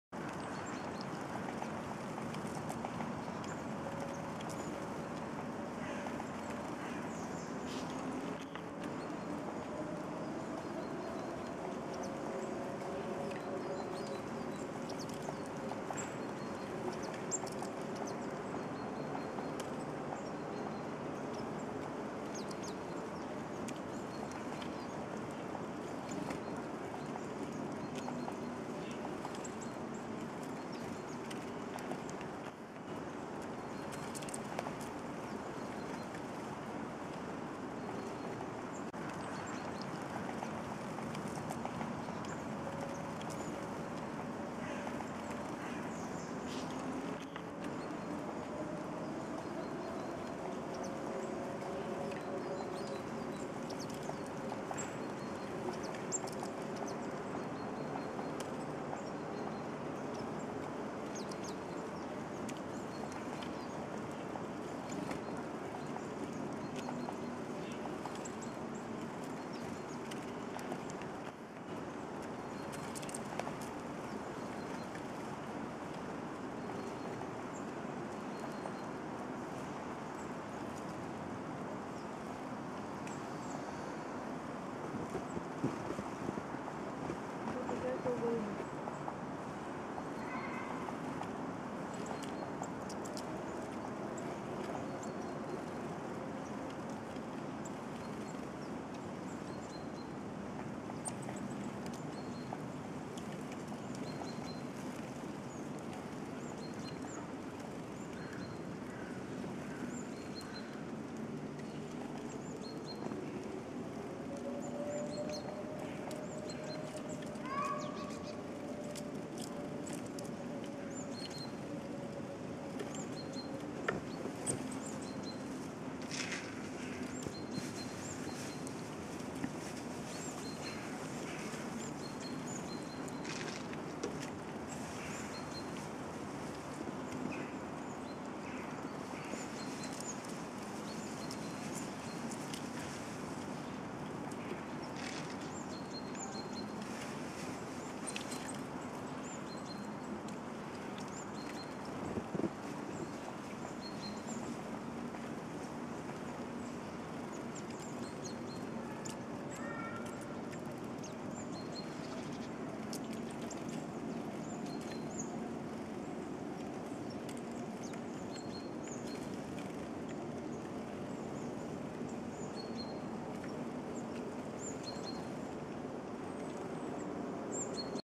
Стайка изголодавшихся лесных птах
Синицы, поползни слетелись на любимое лакомство - семечки в кормушке